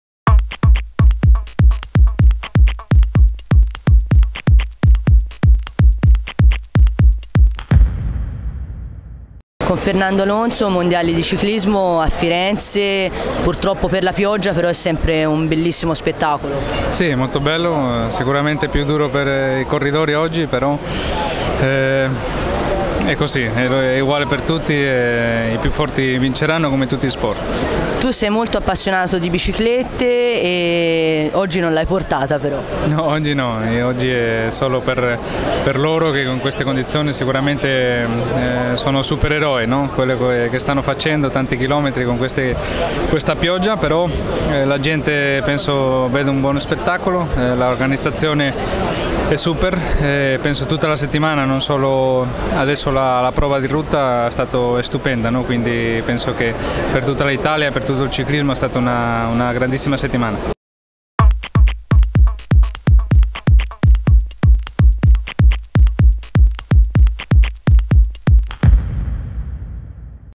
Fernando Alonso a Toscana2013
Fernando Alonso ai Mondiali di Ciclismo Toscana2013. L'intervista